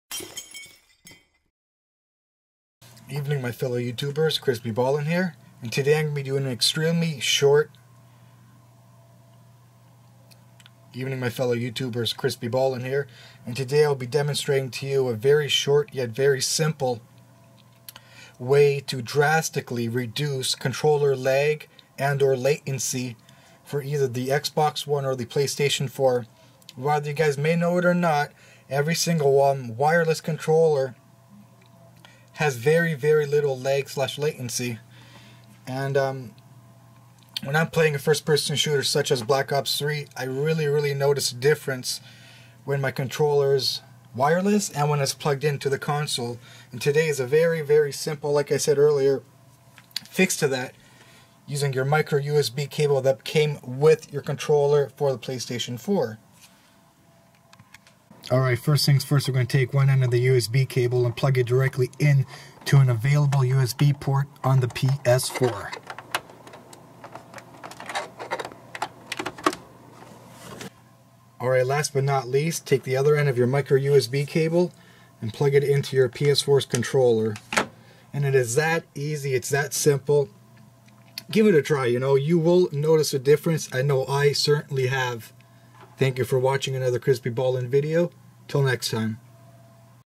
Eliminate / Reduce PS4 Controller Lag / Latency (2 Simple Steps)
I show you how to drastically reduce and eliminate controller latency and or lag. You can really notice the difference when playing a fast paced FPS game. This method will also work on the XBOX ONE console.